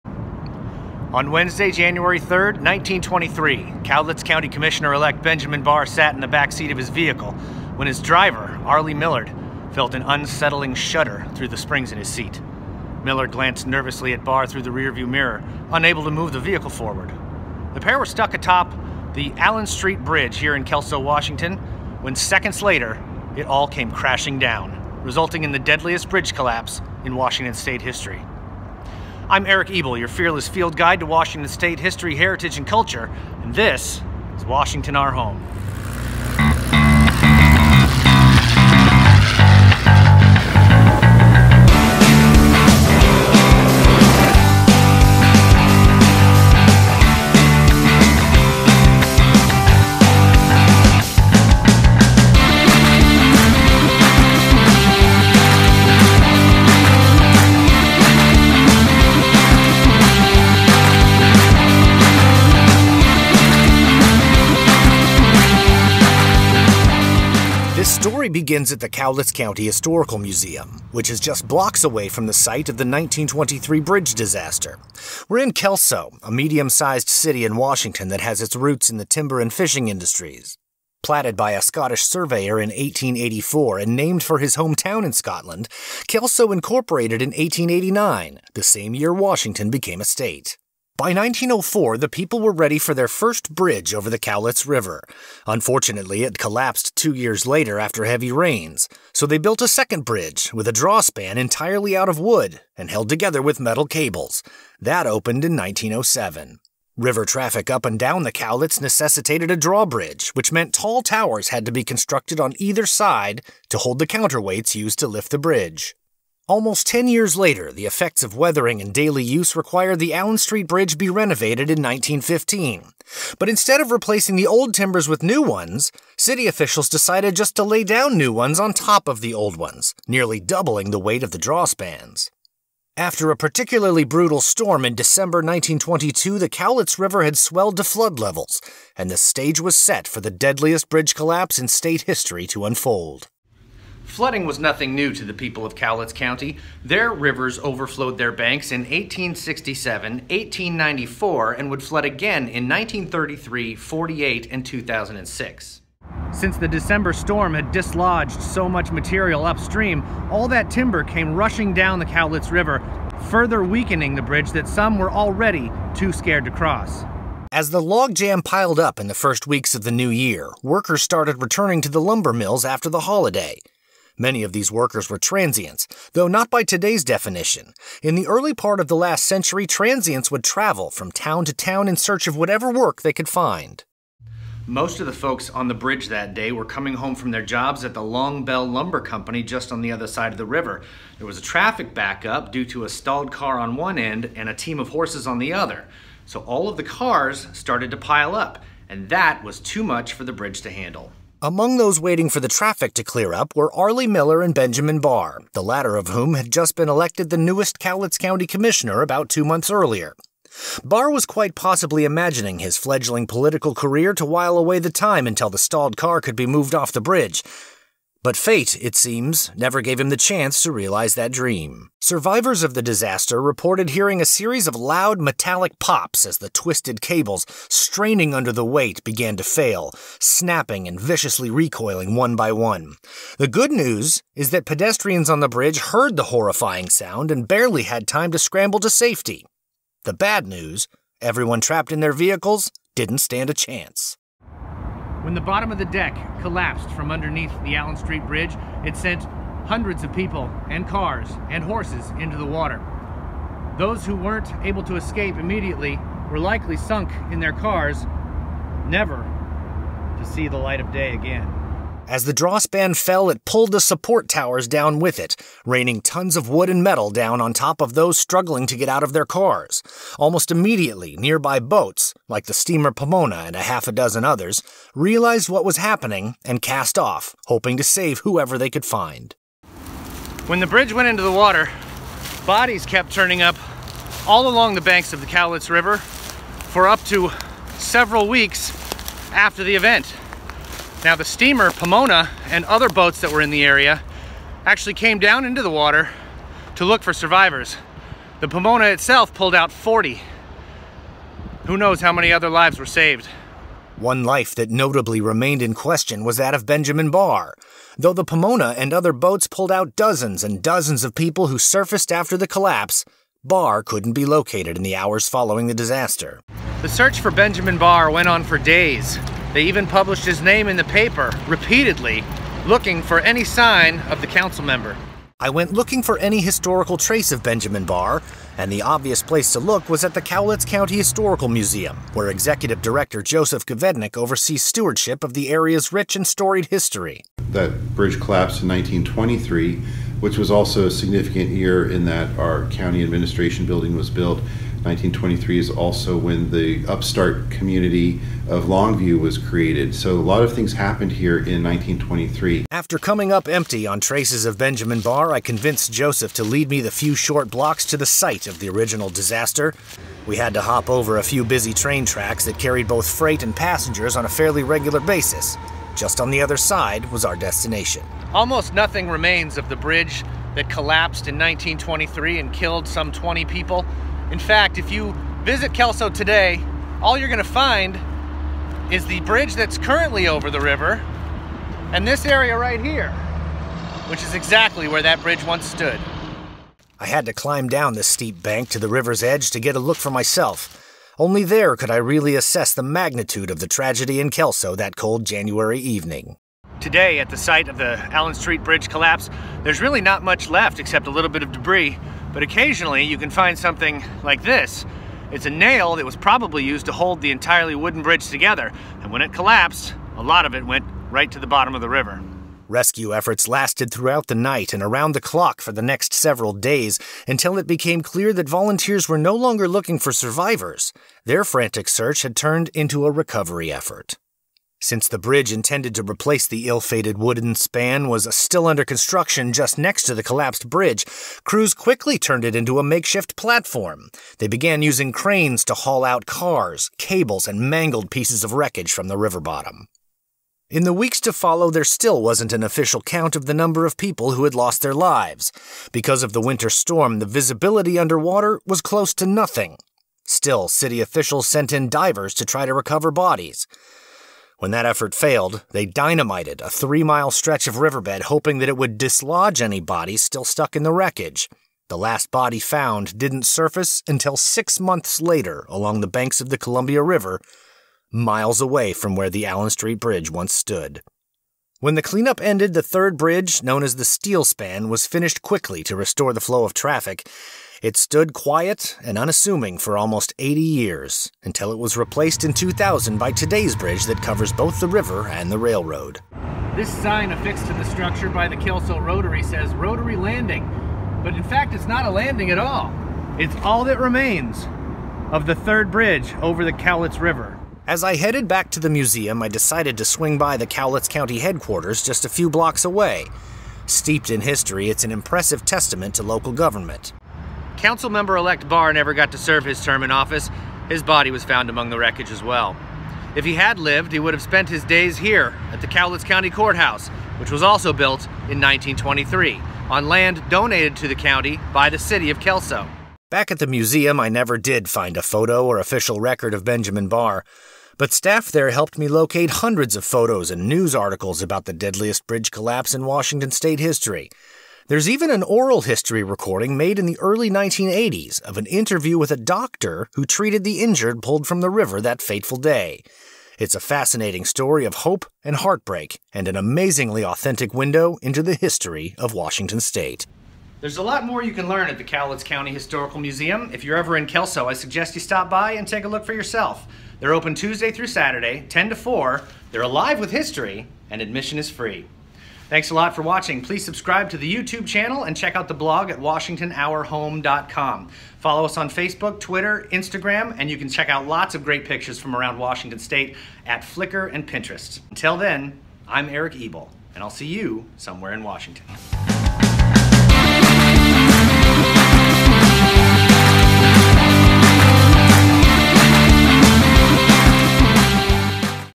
Deadliest Bridge Collapse in Washington State History
On Wednesday, January 3rd, 1923, Cowlitz County Commissioner-Elect Benjamin Barr sat in the back seat of his vehicle, when his driver - Arleigh Millard - felt an unsettling shudder through the springs in his seat. Millard glanced nervously at Barr through the rear-view mirror, unable to move the vehicle forward. The pair were stuck atop the Allen Street Bridge in Kelso, Washington, when seconds later it all came crashing down, resulting in the deadliest bridge collapse in Washington State History.

Photos: The Daily News, Cowlitz County Historical Museum, Joseph Govednik, city-data.com, RomTec Utilities, Inc., R. A. Long Historical Society, Cowlitz County Superior Court, Clackamas County Historical Society, Cowlitz County, City of Kelso